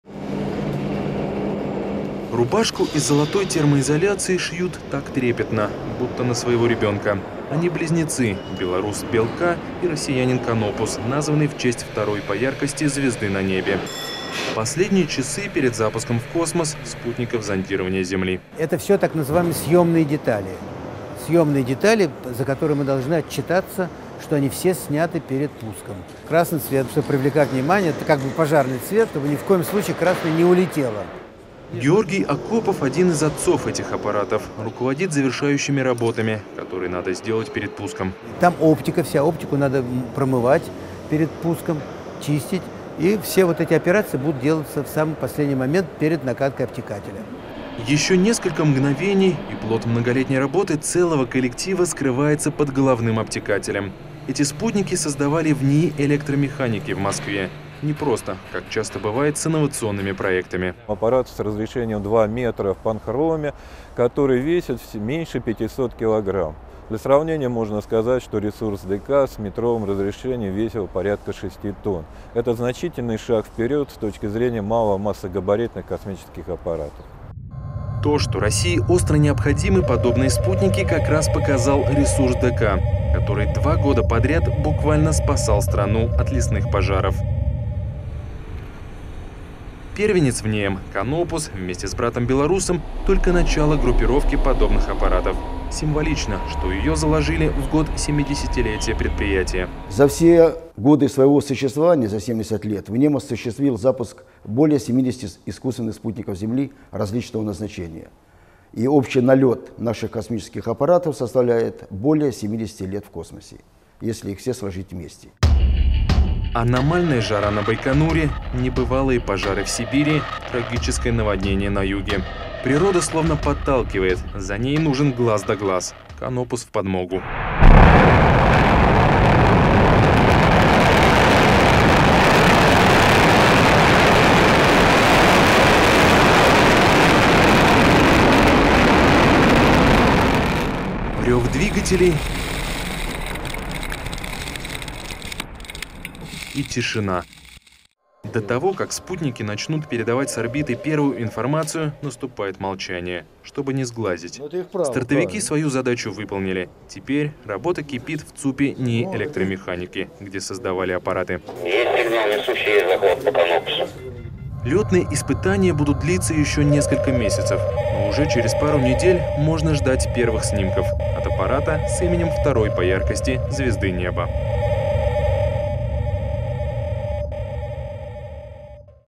Близнецы на орбите
В эти дни российская ракета «Союз» вывела на орбиту первый белорусский спутник.  Одновременно с ним в космос полетел  и российский аппарат «Канопус».  Оба спутника дистанционного зондирования Земли построены в Москве, во ВНИИ электромеханики. С запуском на орбиту  спутников-близнецов Россия и Беларусь создают в космосе группировку, которая будет работать в единой космической системе.